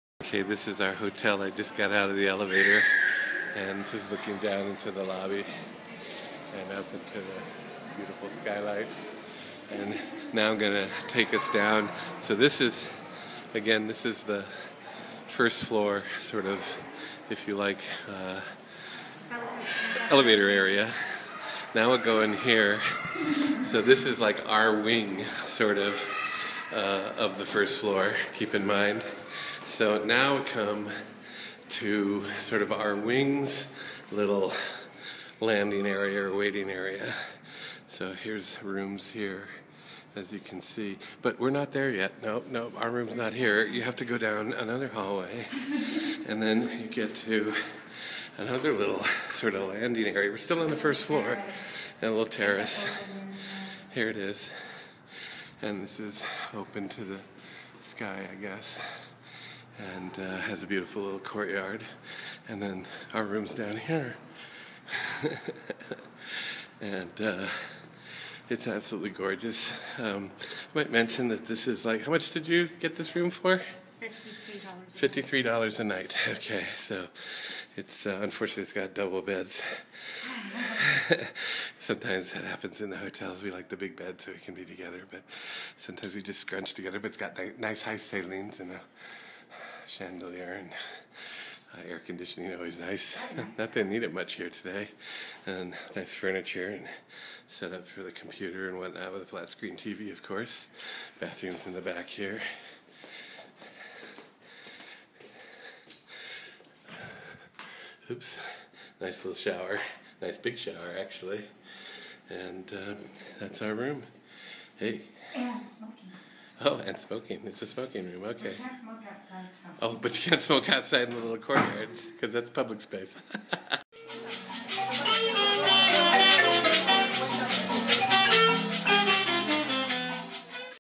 Hotel Morales, Guadalajara
A quick tour of our hotel in Guadalajara. It was originally a mansion, but now a hotel. Each floor has several beautiful public courtyards for sitting areas, some with koi ponds or fountains, some with sky lights and all with nice chairs and tables. There are courtyards within courtyards, if that is possible.